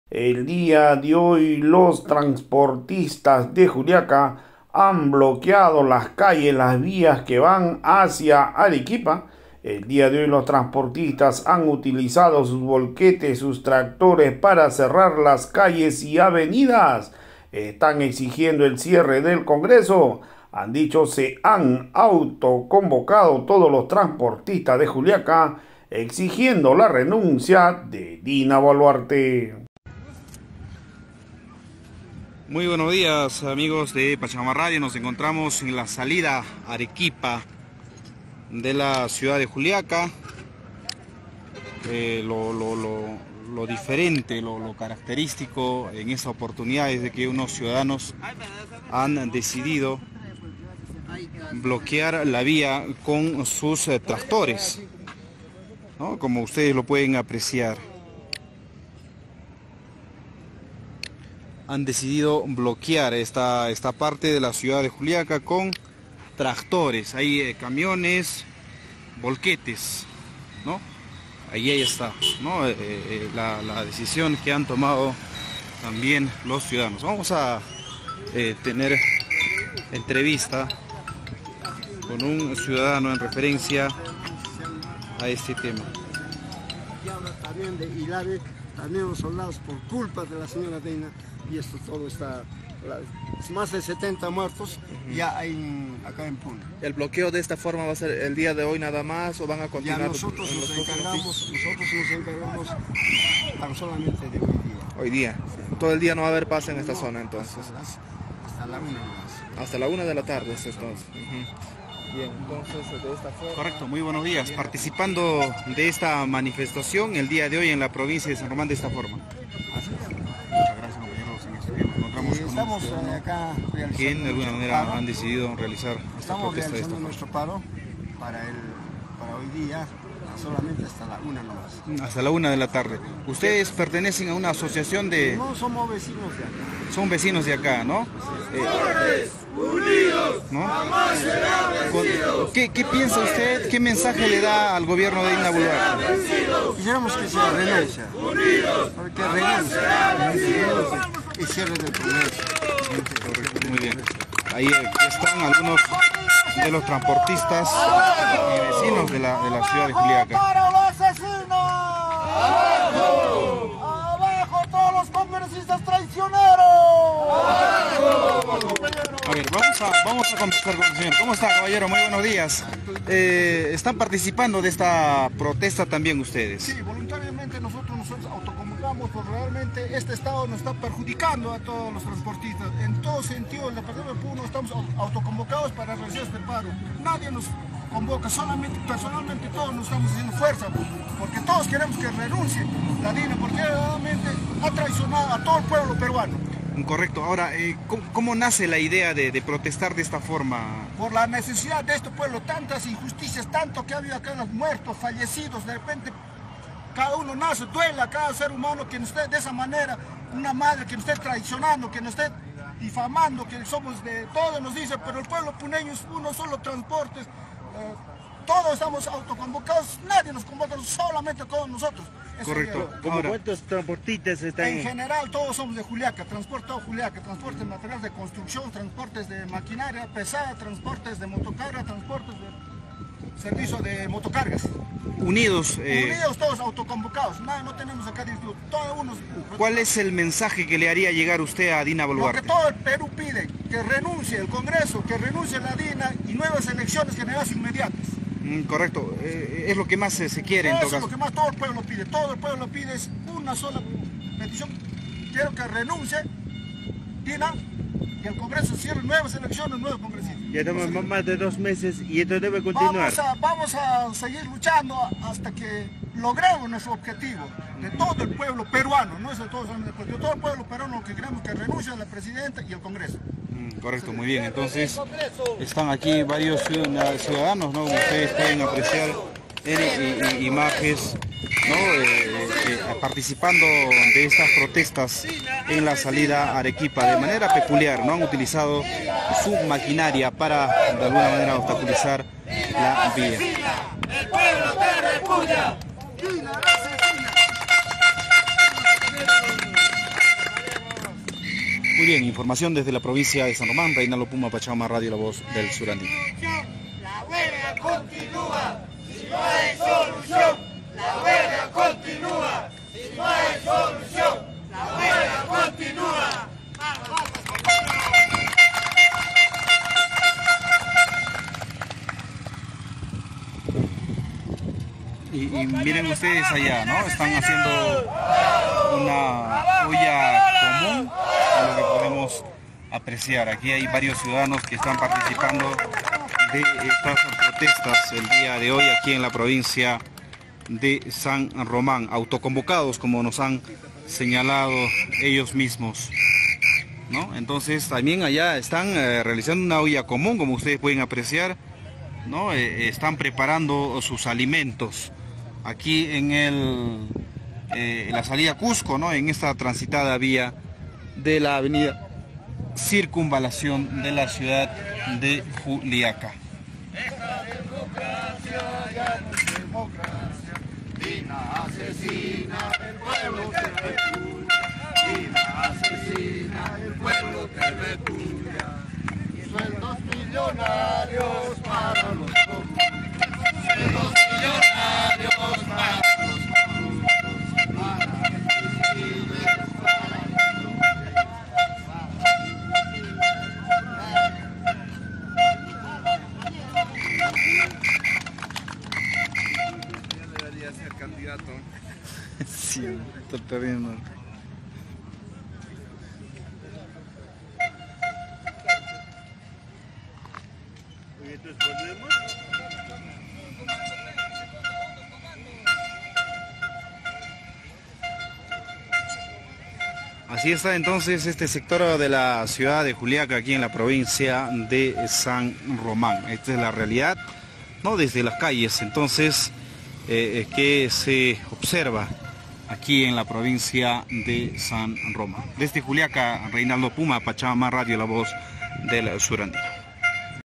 TRANSPORTITAS BLOQUEAN A LA SALIDA DE JULIACA HACIA AREQUIPA..
BLOQUEAN CON VOLQUETES Y TRACTORES PIDIENDO LA RENUNCIA DE DINA BOLUARTE..